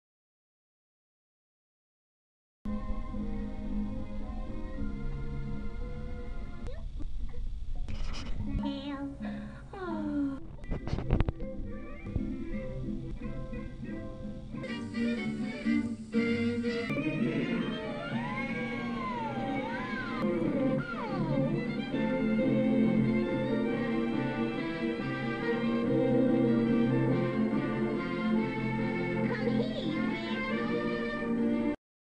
mind controlled audio "accident"
joke about a singer throwing a microphone

I was recording found footage for an animation project.
Im under surveilance / being gang-stalked
so the stalker types point me at the videos they'd like me to record
before I get there theyve planned different audio or visual ideas and control my hand or cause my finger to cover the mic or possibly inject audio into the camera . Then as I work on the edit of the animation in my studio they try to contol my work through suggestion and mind control . Same deal for just about any person on tv or any writer youve heard of.  so um and you no